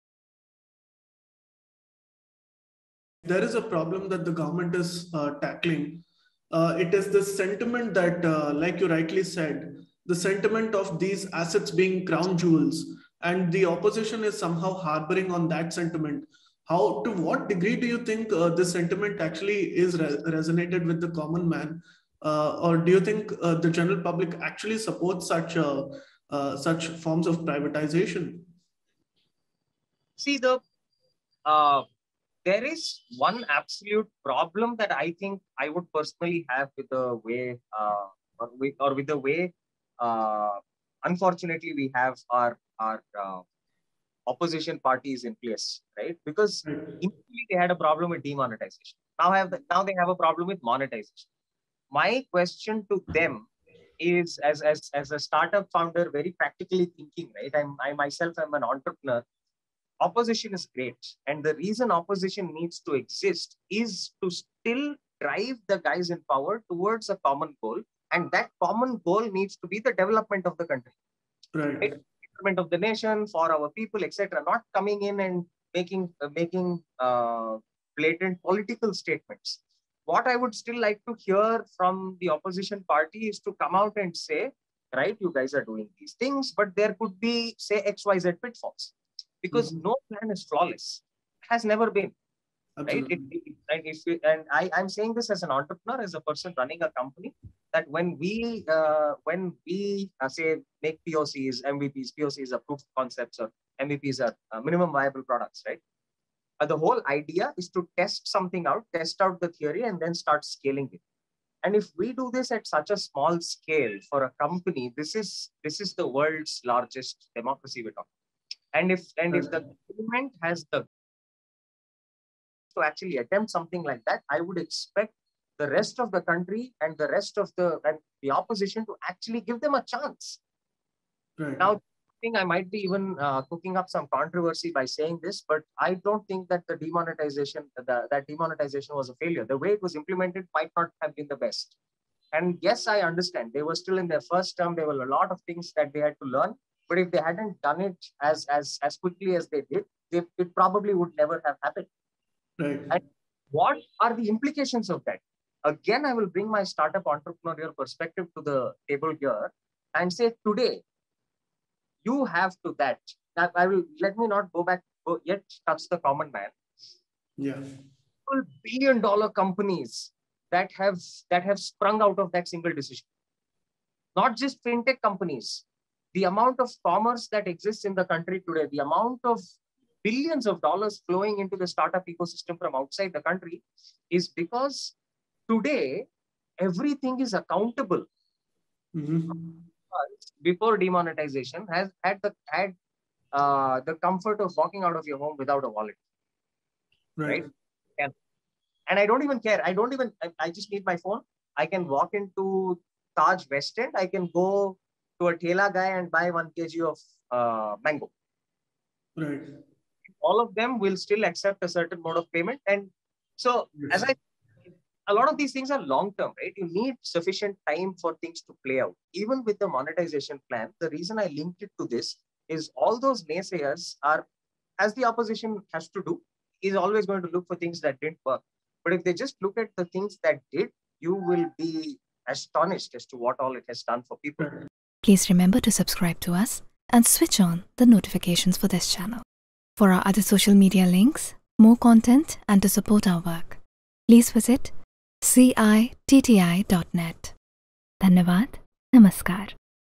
"Demonetisation hurt me yet I fully support it." Gotama Gowda
"As an entrepreneur, I was affected by demonetisation, yet I fully supported it." Gotama Gowda explains why